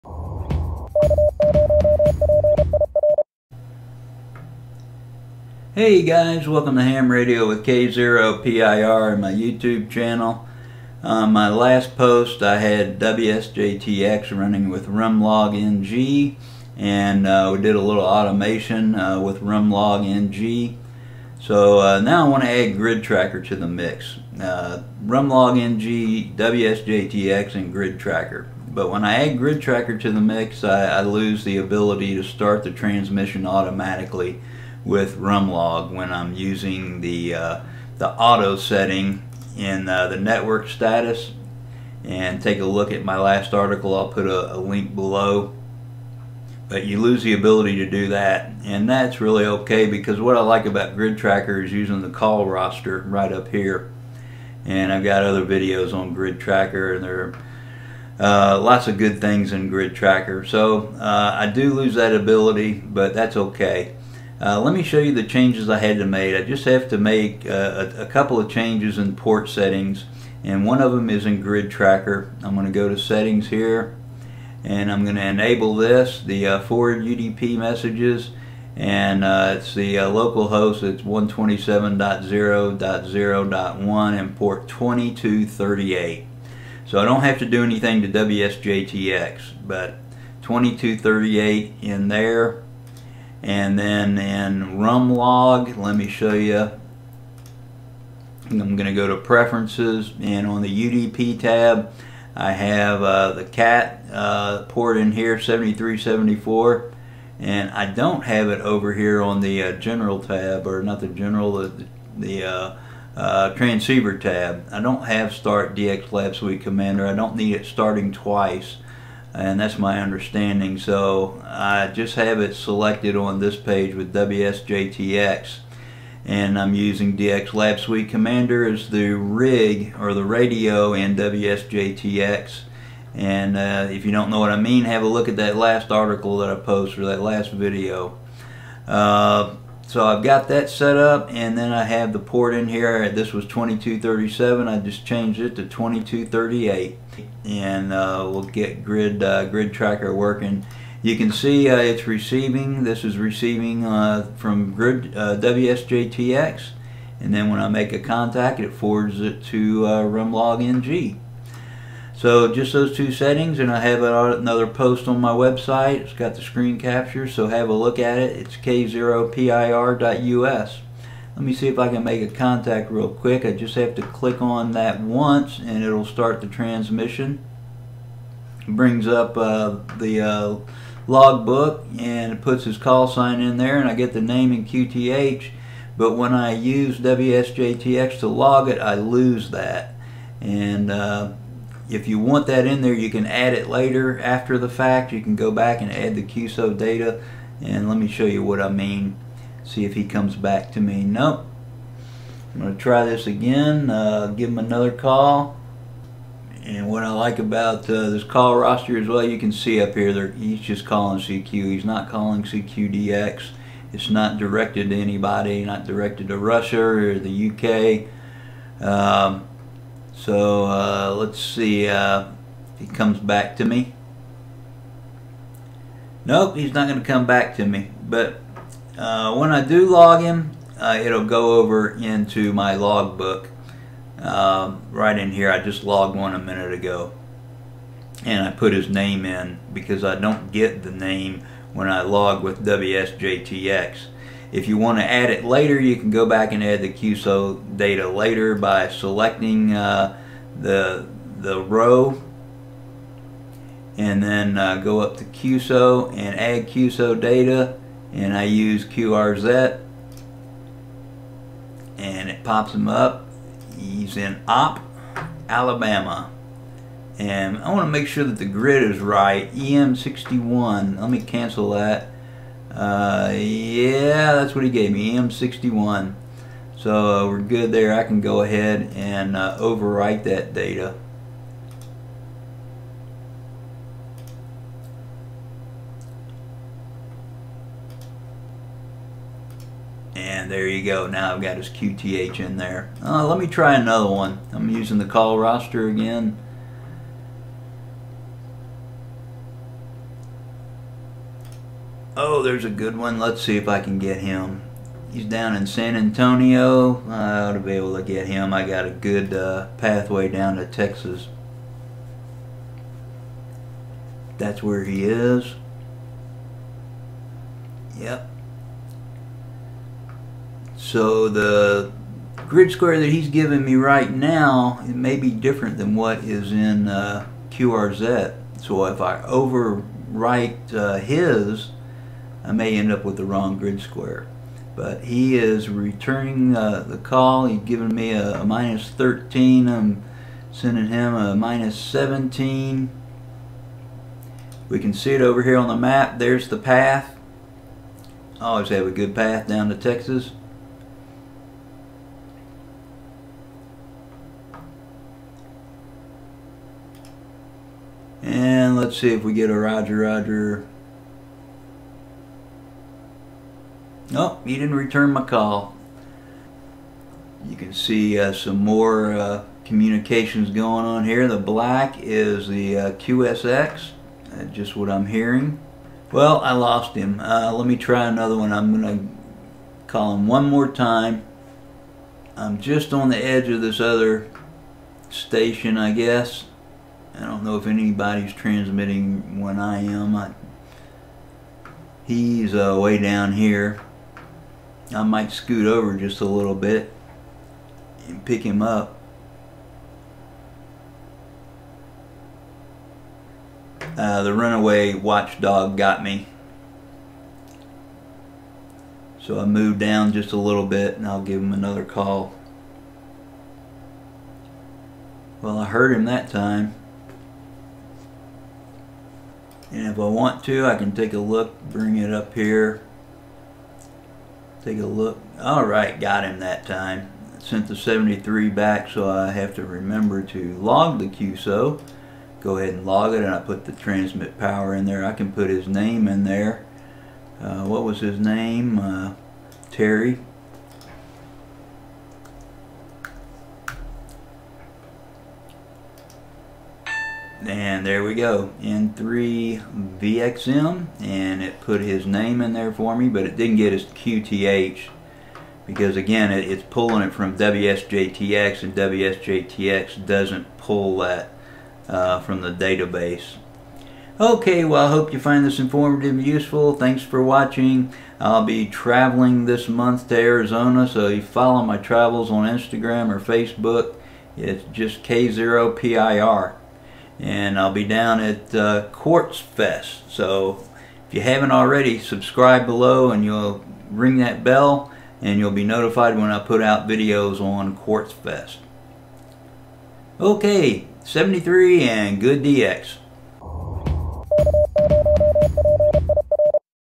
RUMLogNG WSJT-X and GridTracker - MacOS Apple M1
GridTracker is a great addition to WSJT-X and I'm using RUMLogNG for my logging program.

○○○ LINKS ○○○